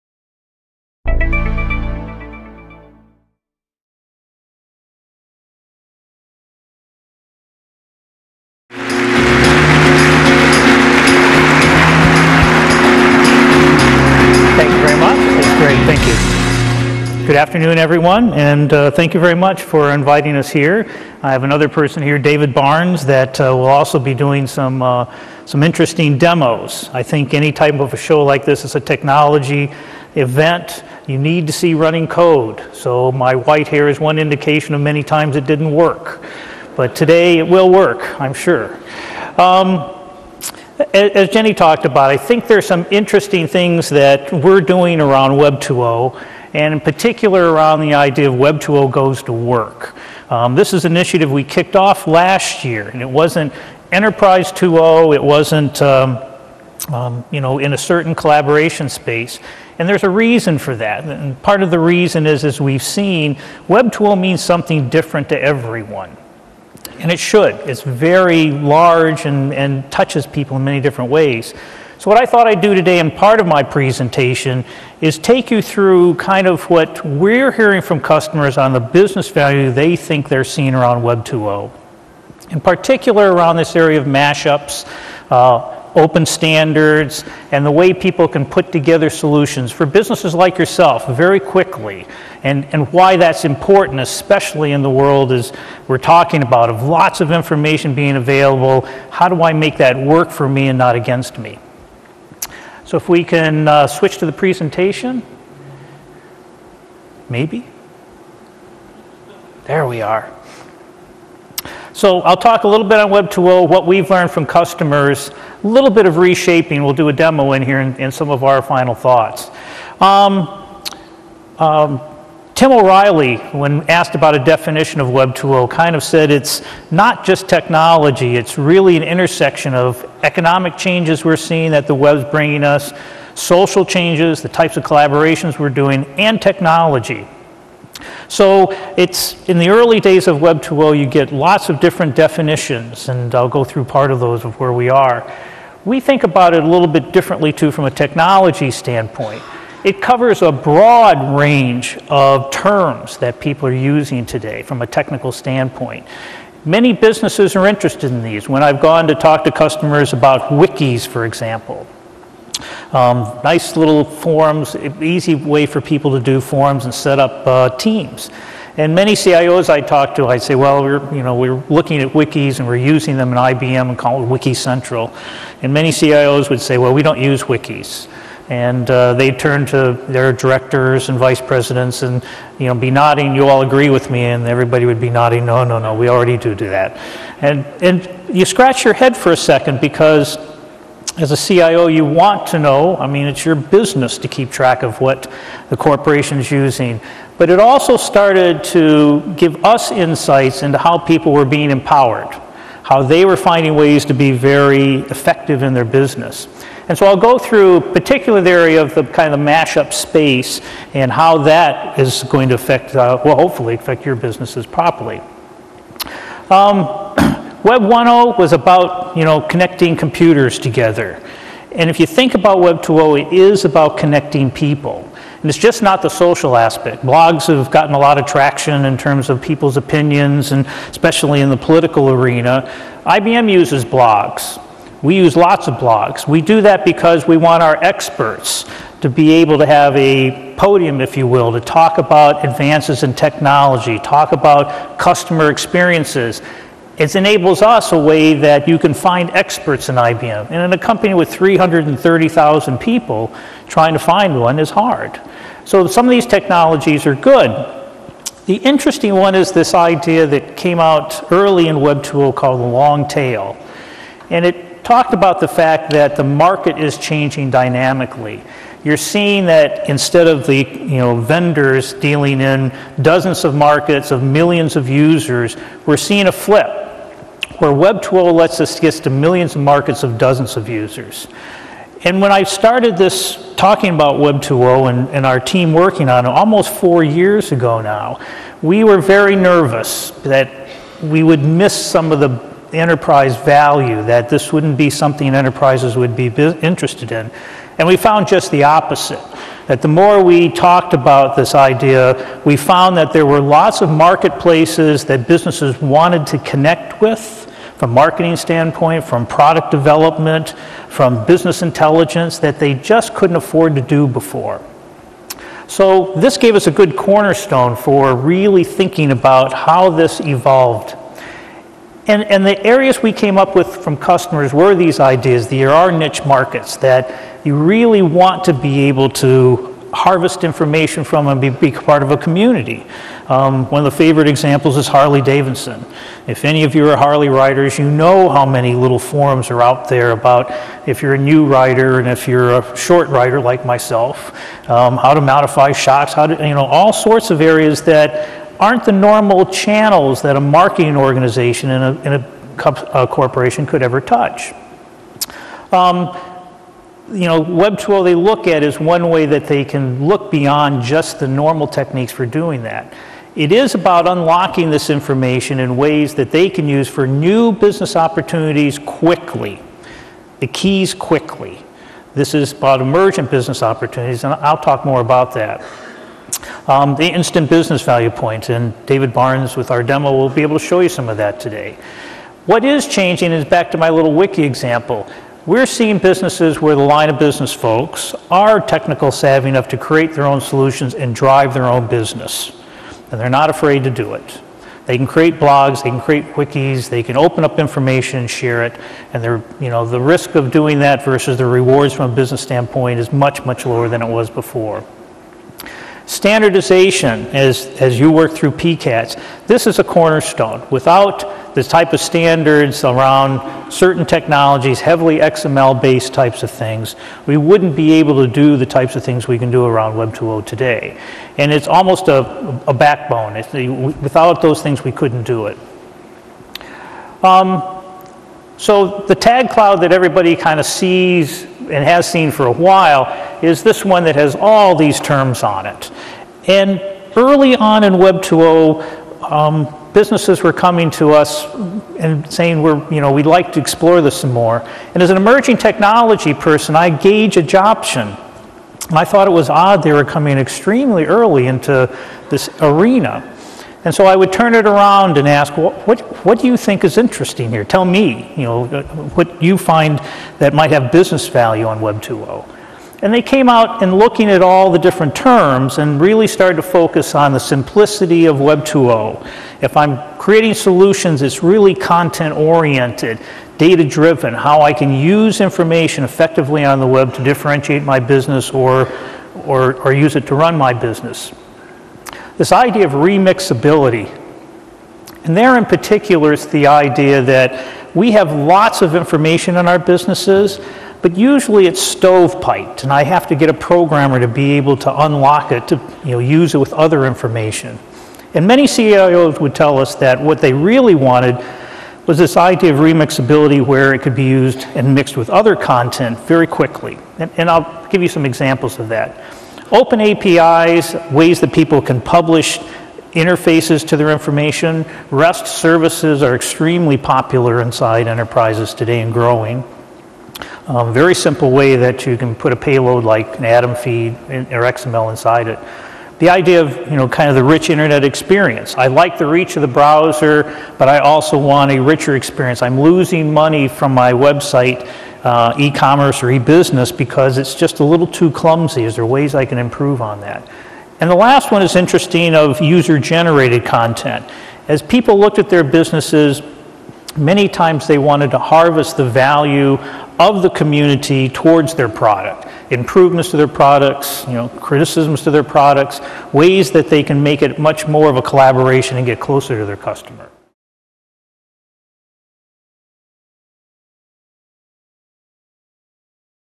Rod Smith on Mashups Pt. 1
Rod Smith of IBM discusses the use of Web 2.0 style mashups in business. Part one of a four part series.